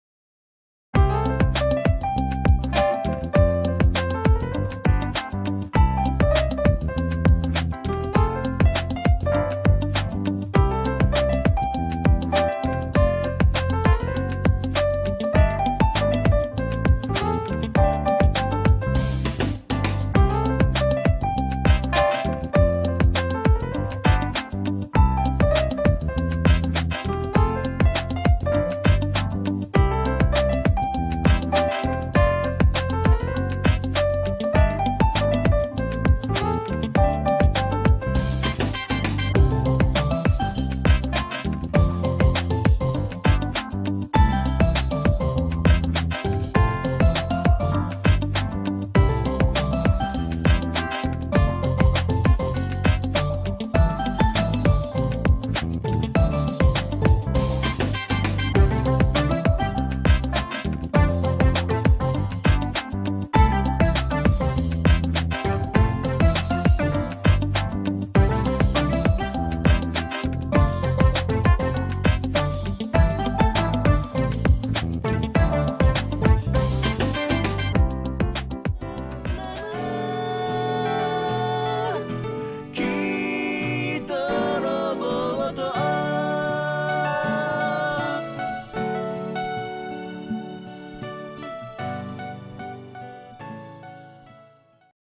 オススメ仙台グルメ13◆餃子と担々麺「吟」(パスタみたいなオシャレなラーメン)
美味しいラーメン屋さん、宮城おすすめラーメン